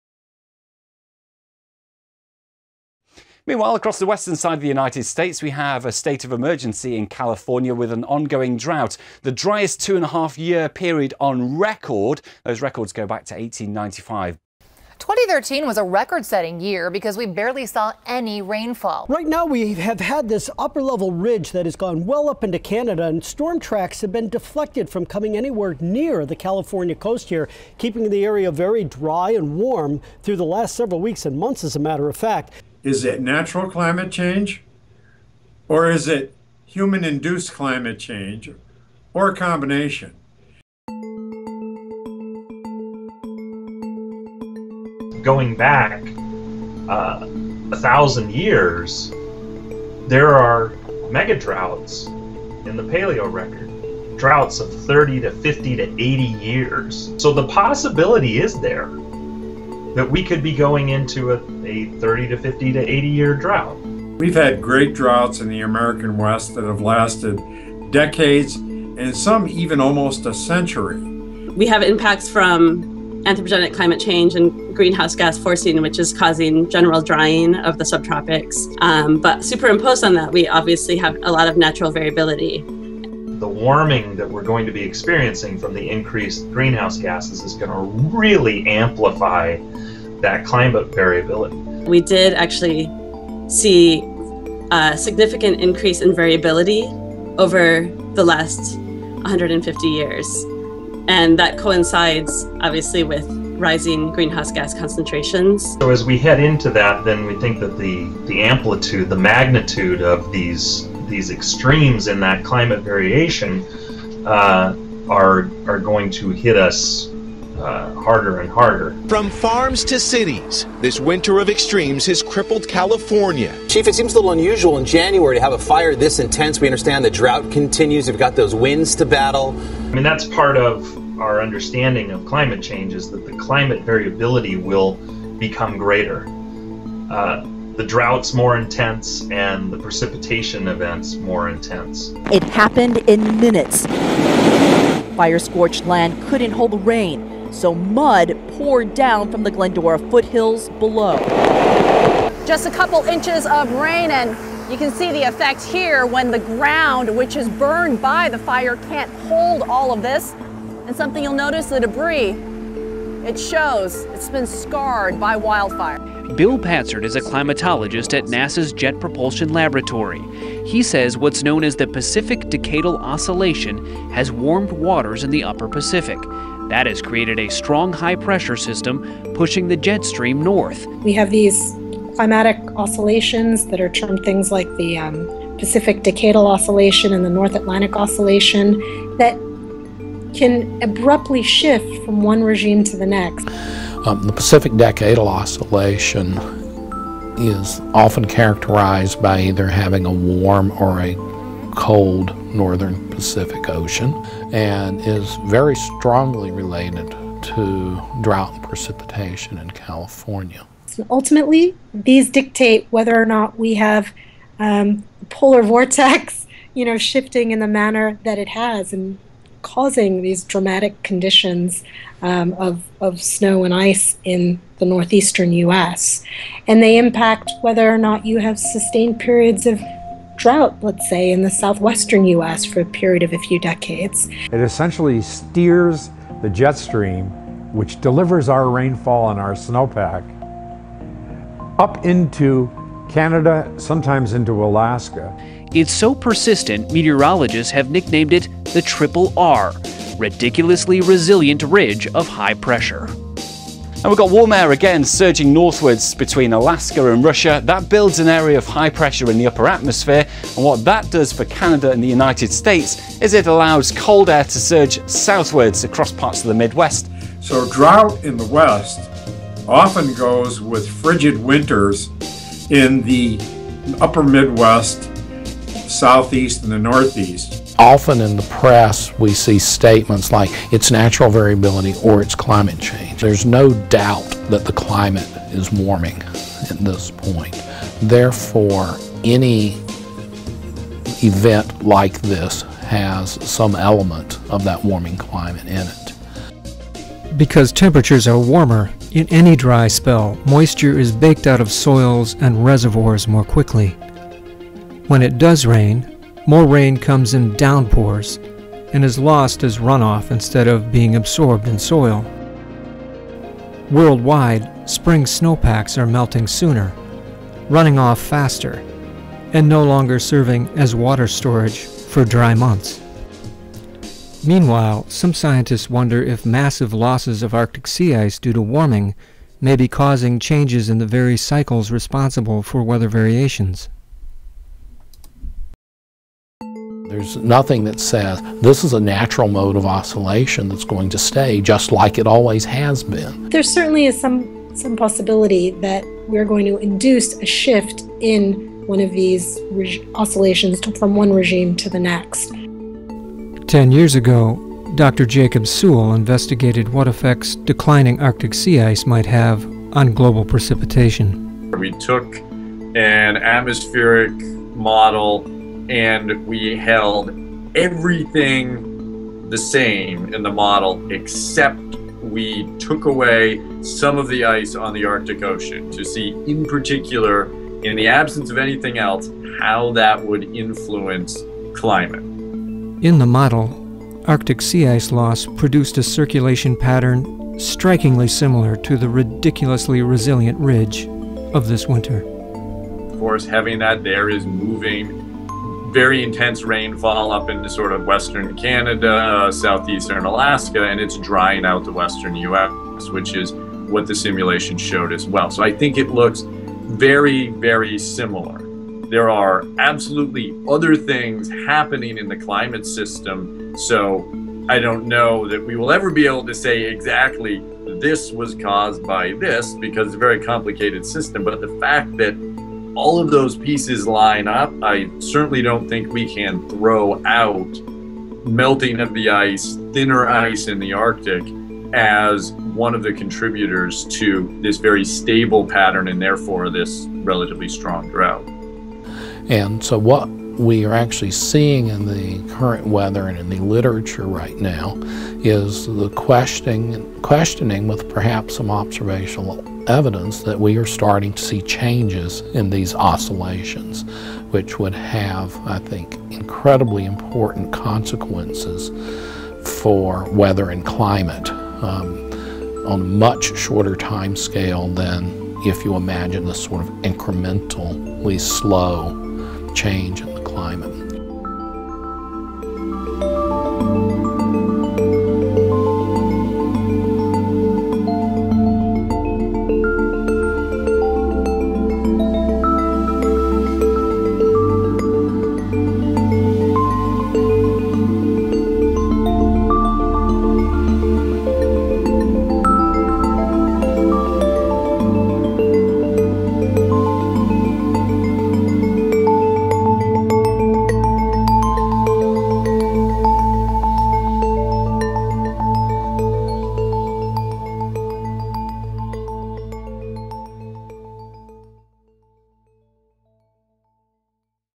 California Drought, and Climate Change
Scientists weigh in on California drought. If you've been wondering where I've been --- you've been missing a lot of good videos!!!